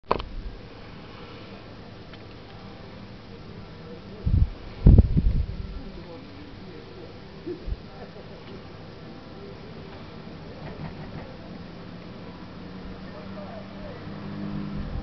Endlich schneit es bei uns!
Pünktlich zum 1. Advent schneit es nun auch bei uns!